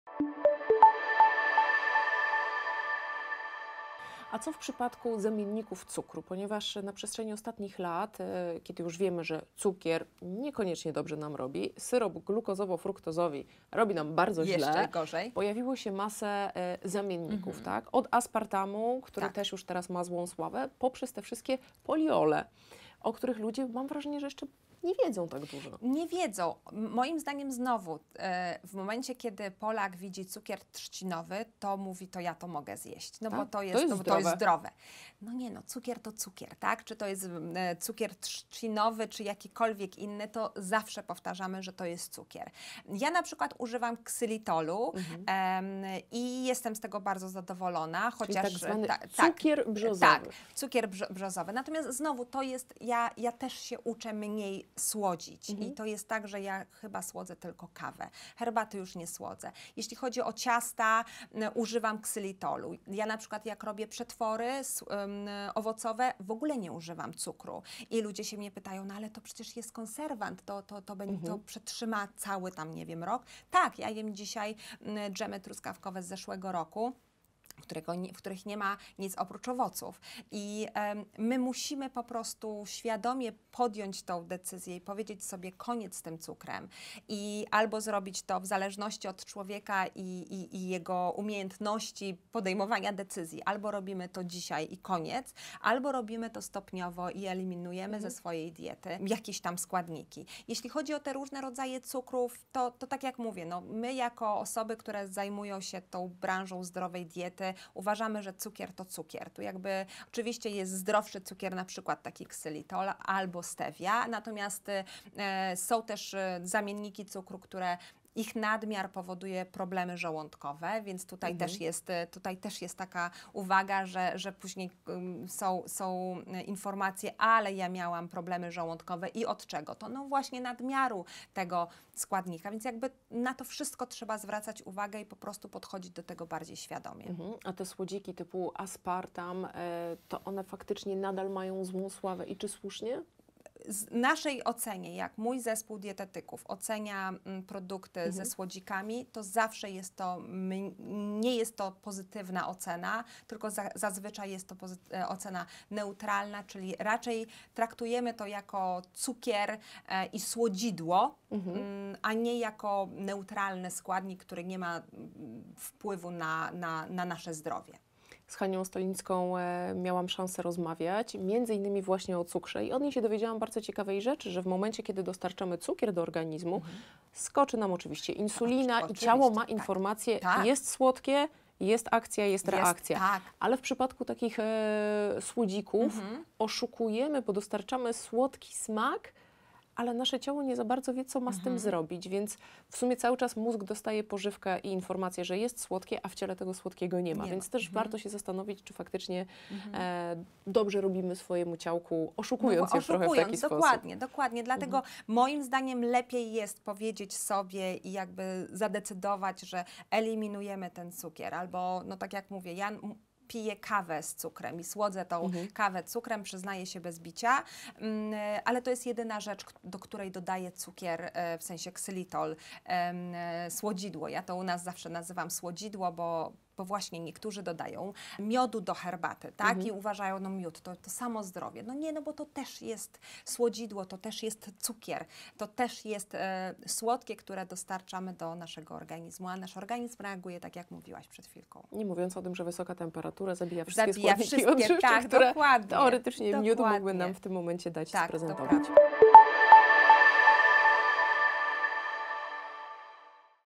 Miód, ksylitol, a może stewia? Agnieszka Pocztarska mówi, czym najlepiej zastąpić cukier
Jeśli nie cukier... to co? Zamienników cukru jest kilka, od nas zależy, na co się zdecydujemy. Czym się różnią między sobą?  I czy rzeczywiście lepiej słodzić kawę miodem albo cukrem trzcinowym? Jakich słodzików warto używać? Wyjaśnia Agnieszka Pocztarska, właścicielka portalu Czytamy Etykiety.

0:35 - cukier biały kontra cukier trzcinowy
1:48 - zamienniki cukru
3:46 - miód zamiast cukru to dobry pomysł?